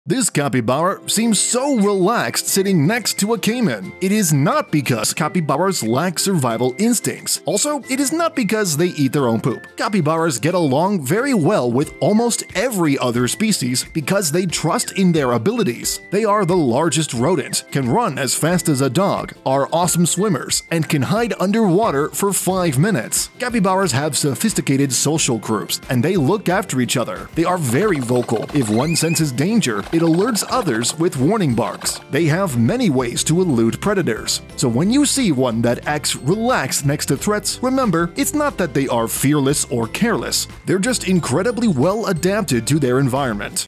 The Secret of Capybara's Fearless Behavior
Ever wonder why capybaras seem unfazed around threats? Hint: it's not just their fearless attitude! This video takes a wild ride into the capybara's world. They're more than just the largest rodents around, they've got some mad skills that help them survive. Speed? Check. Swimming? Absolutely. Evasive strategies? You bet! So dive in with us, you'll never look at a capybara the same way again!